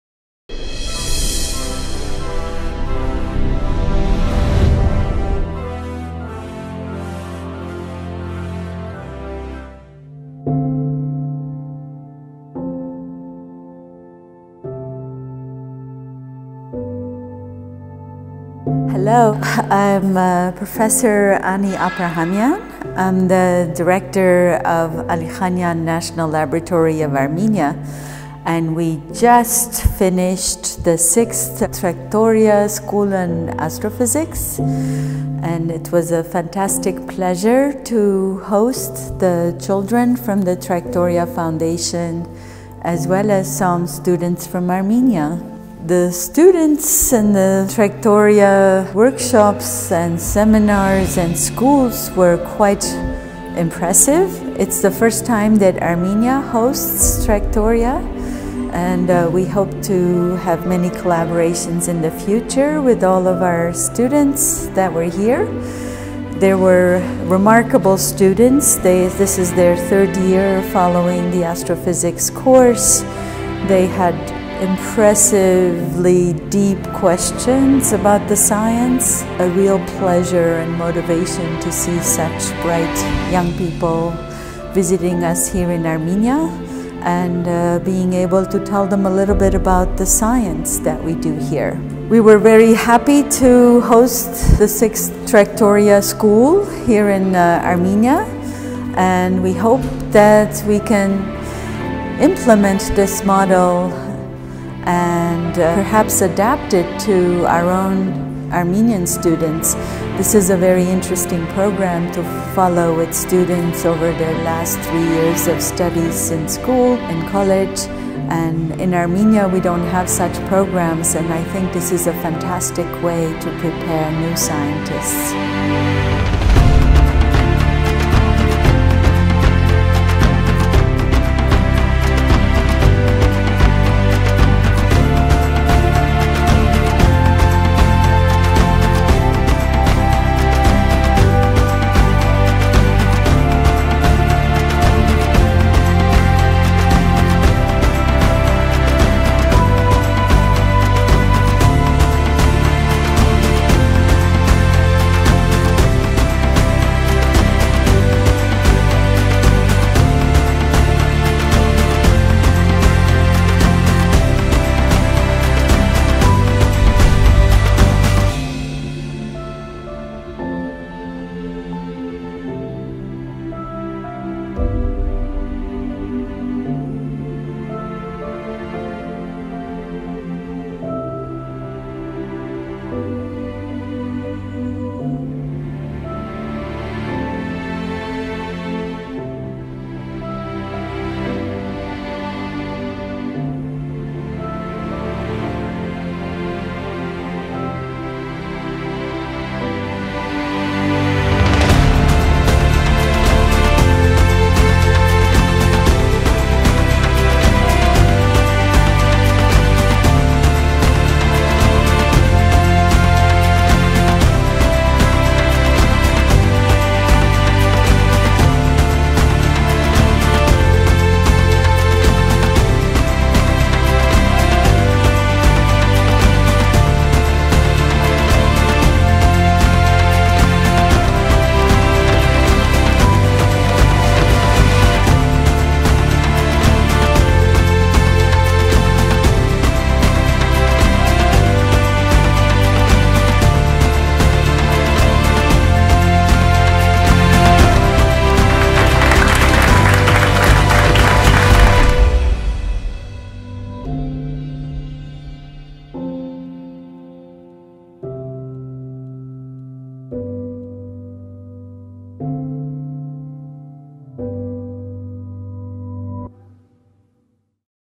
6-я Астрофизическая школа "Траектория" в Ереване
Ролик снят во время 6-й Астрофизической школы "Траектория", которая проходила в партнерстве с Национальной научной лабораторией Армении им. А. Алиханяна (Ереванским физическим институтом) и Бюраканской астрофизической обсерваторией им. В.А. Амбарцумяна.
Армения, 25 марта – 3 апреля 2019 года.

В видео участвуют наши студенты, преподаватели, сотрудники  Национальной научной лаборатории Армении им. А. Алиханяна и Бюраканской астрофизической обсерватории и сотрудники Фонда.